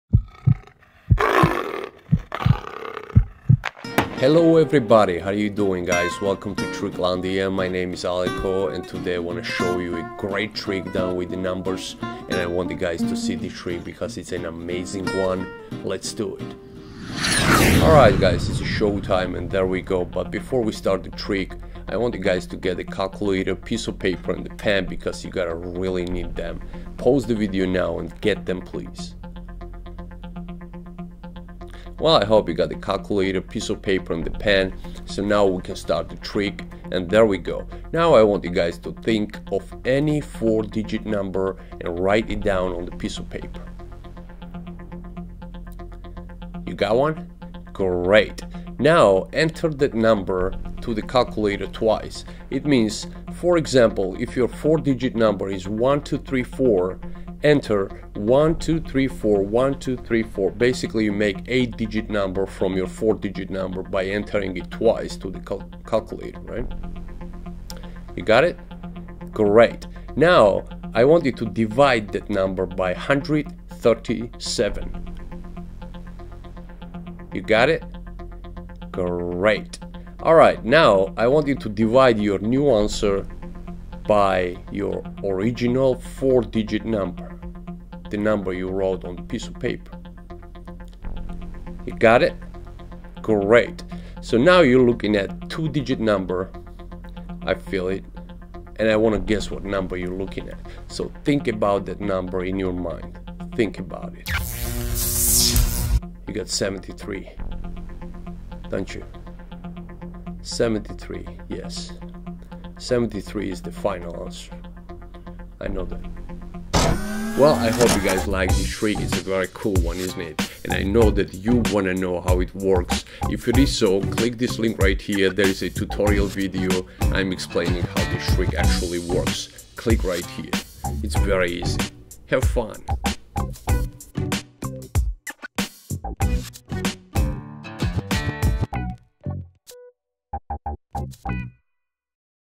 How to Trick Your Math Teacher 6 - Math Tricks Revealed
This is an amazing Math Magic Trick
No preparation needed, can be performed anywhere any time
It's very Easy and Super Effective !
This Trick will make you look like a Math Genius !!!
It can Actually Trick your Math Teacher :)